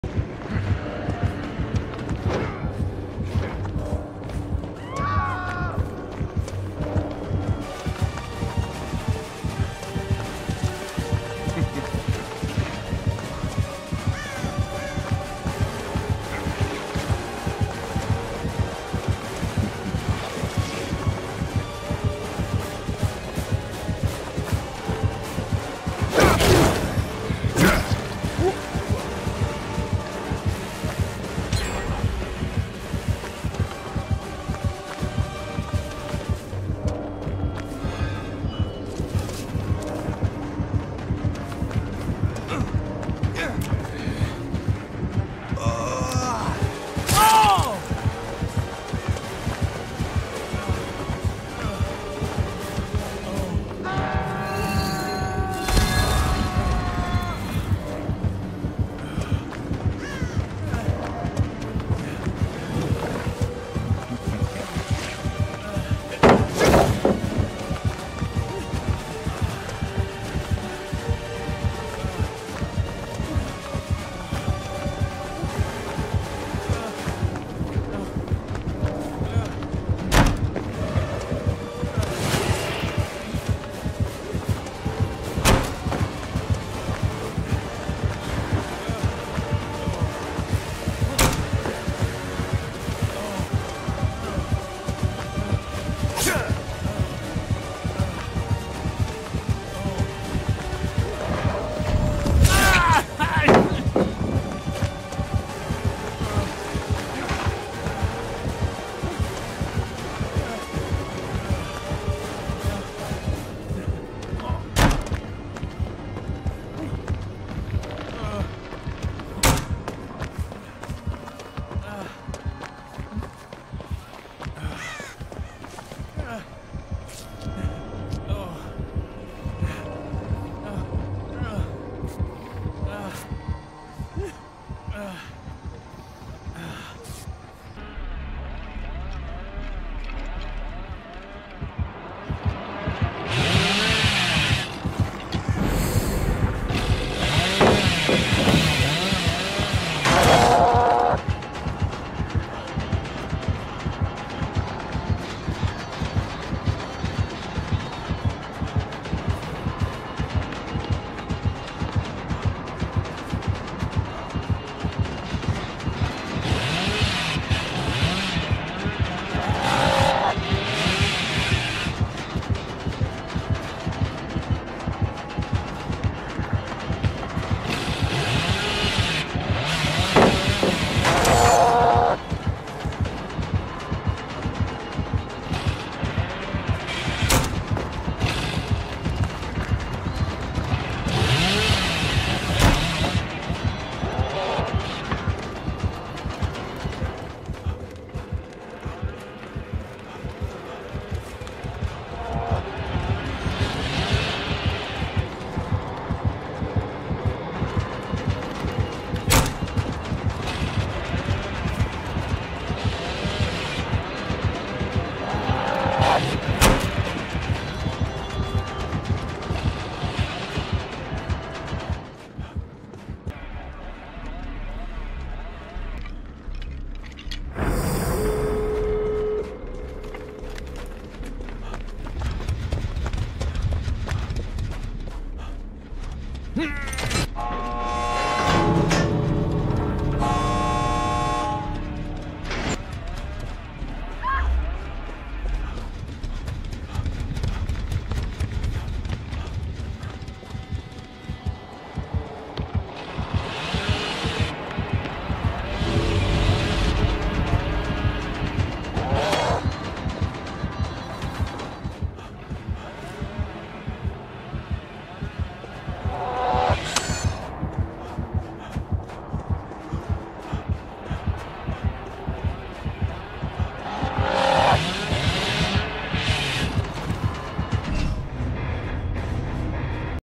Looping Killers Like a Pro
In this video I show a couple clips i got of looping a Wesker and a Billy, pulled off some good plays. :)